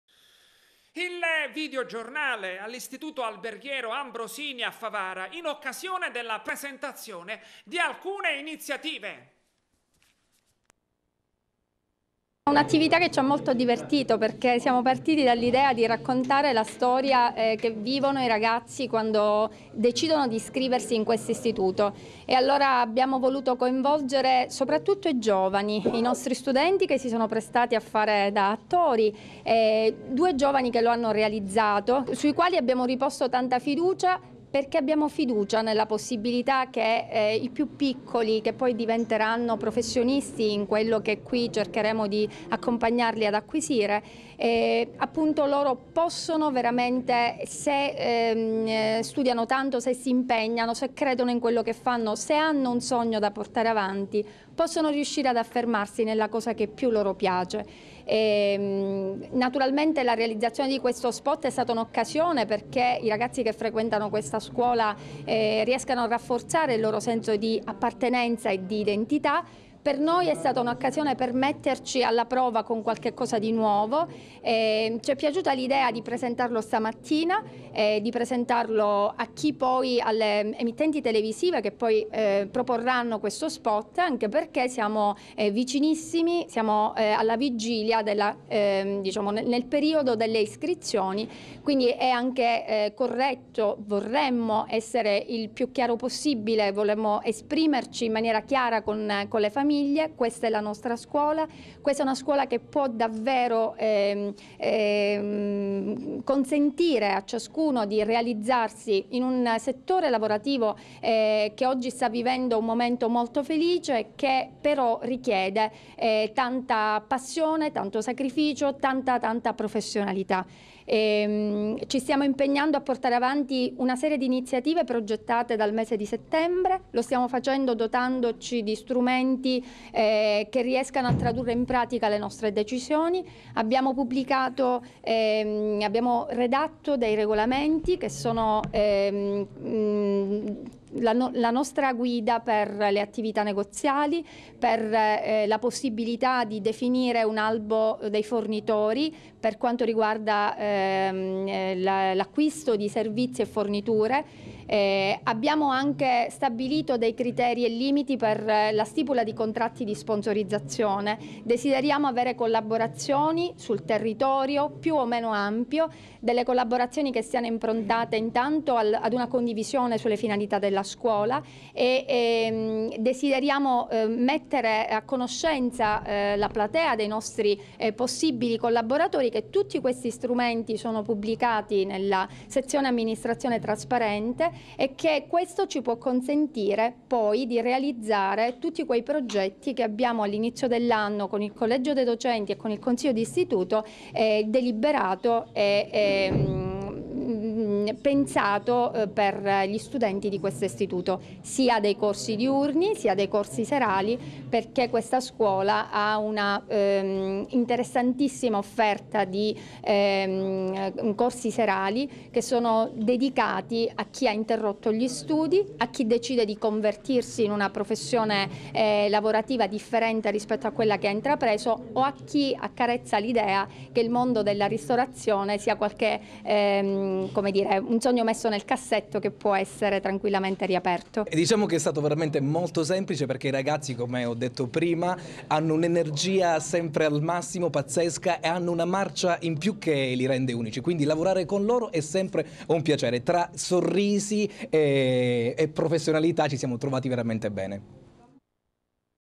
Teleacras - Favara, iniziative all' "Ambrosini"
Il Videogiornale all’ Istituto Alberghiero Ambrosini a Favara, in occasione della presentazione di alcune iniziative. I dettagli nelle interviste.